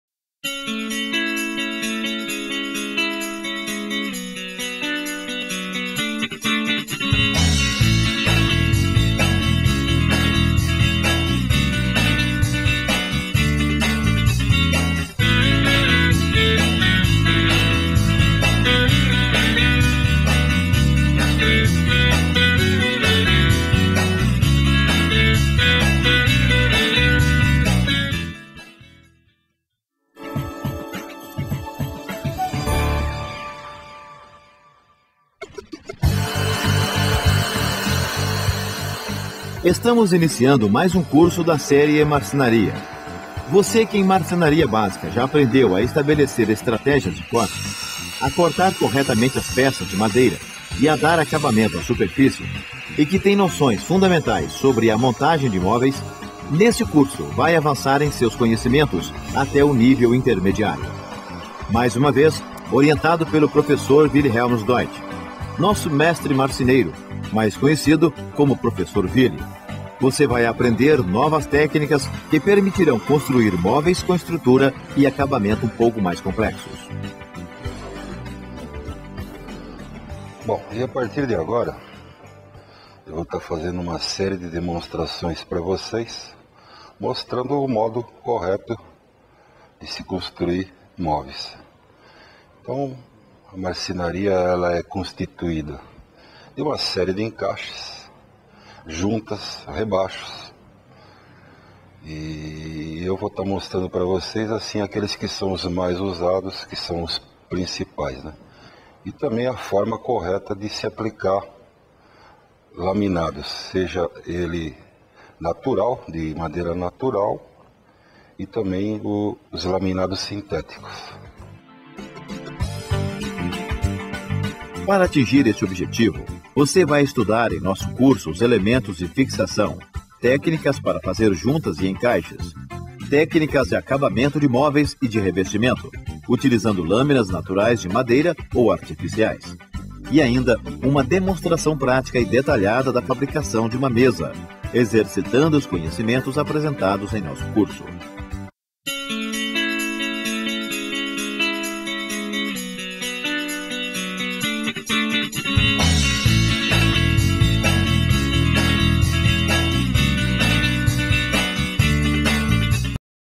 Curso de Marcenaria - Nível Intermediário - Cursos CPT
Este é um demonstrativo do filme que compõe o curso CPT Curso de Marcenaria - Nível Intermediáriol. O Curso completo é constituído de livro com 145 páginas e, em DVD com total de  79 minutos.
O curso aborda: segurança no trabalho, elementos de fixação, juntas e encaixes e laminação.